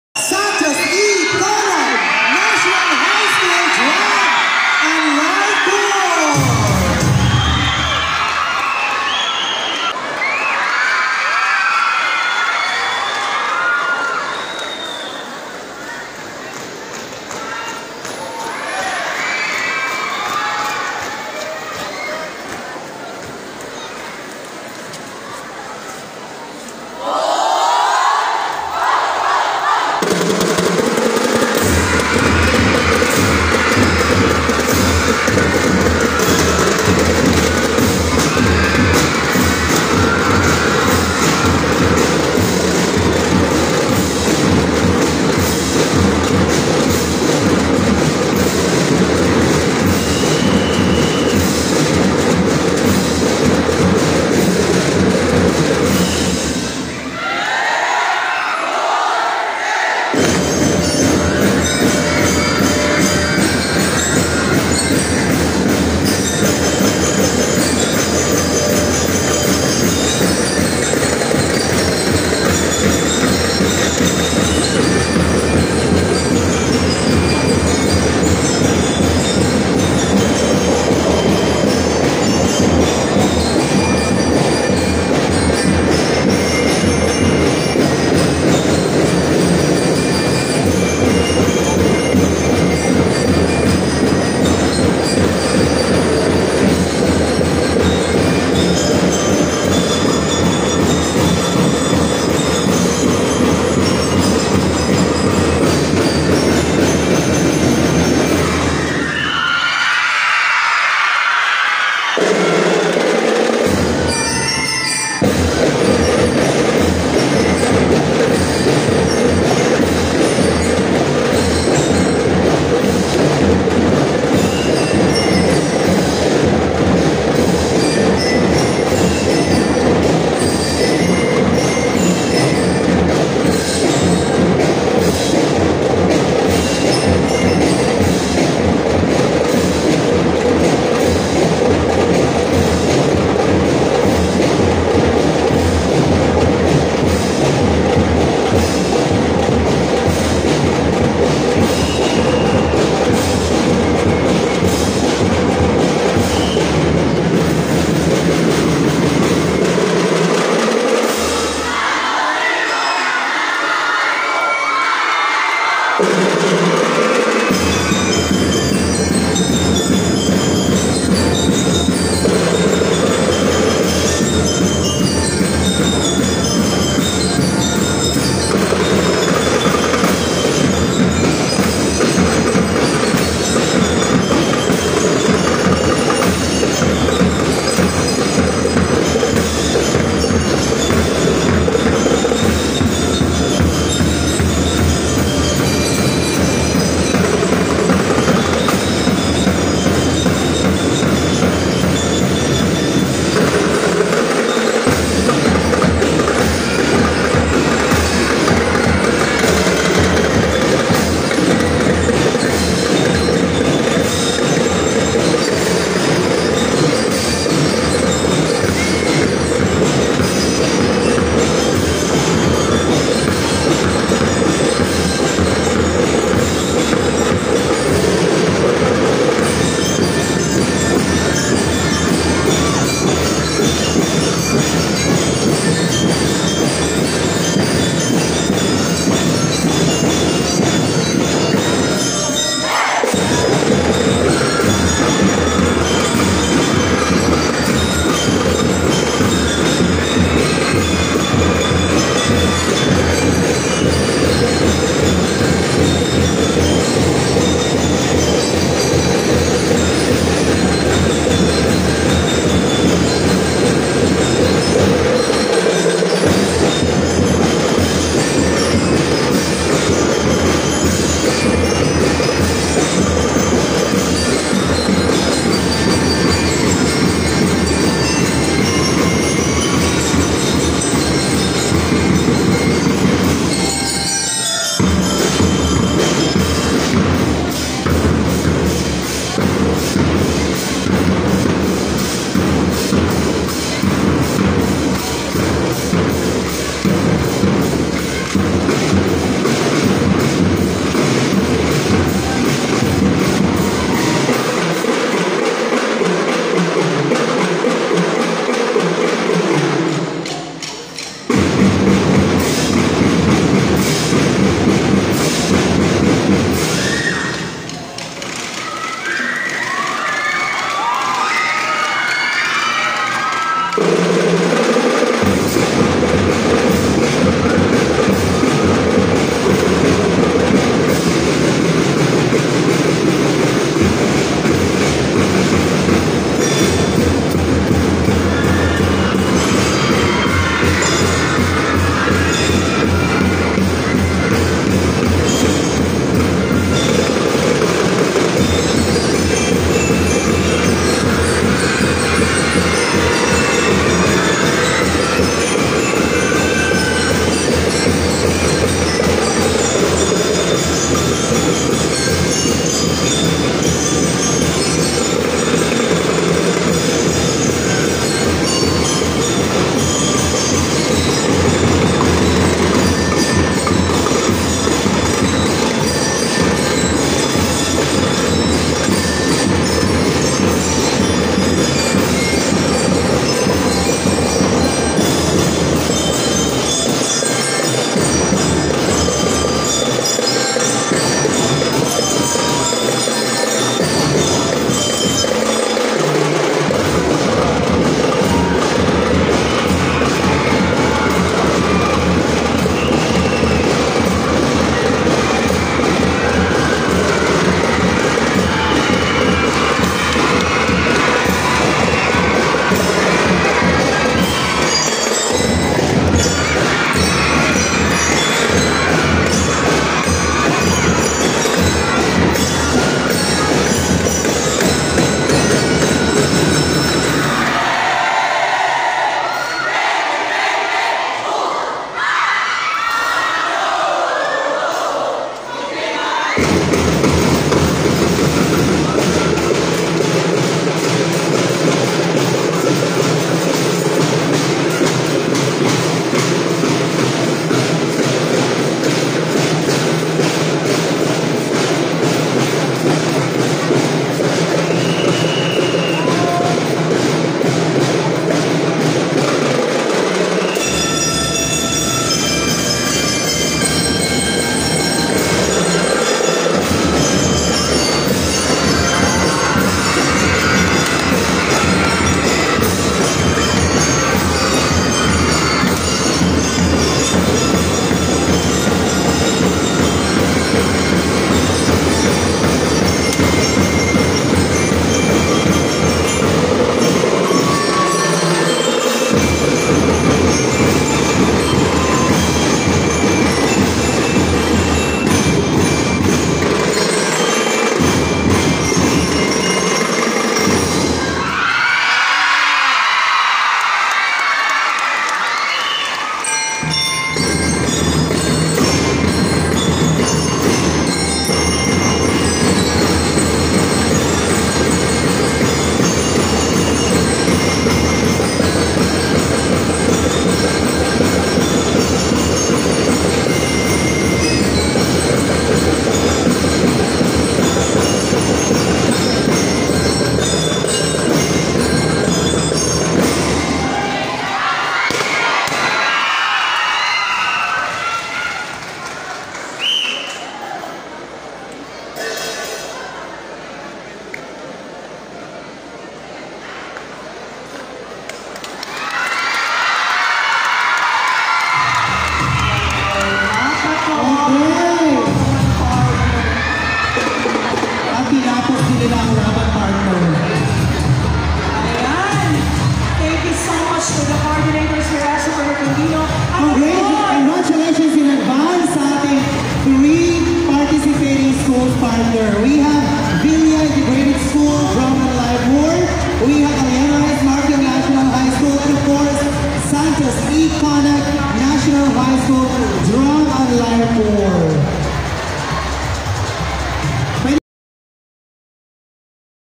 SECNHS DLC | CHAMPION | BATTLE OF THE BAND
Battle of the band overall champion during the Kasadya sa Esperanza 2023.